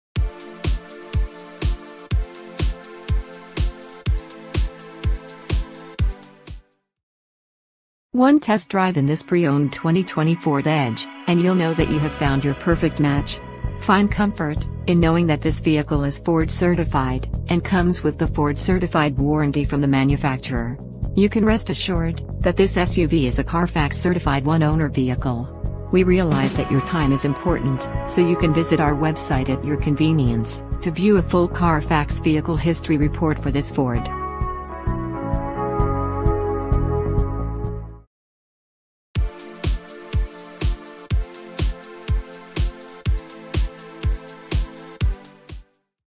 USED 2020 FORD EDGE Titanium at Ferman Ford Clearwater (USED)
This is a USED 2020 FORD EDGE Titanium offered in Clearwater Florida by Ferman Ford Clearwater (USED) located at 24825 US Highway 19 North, Clearwater, Florida

Stock Number: PF2353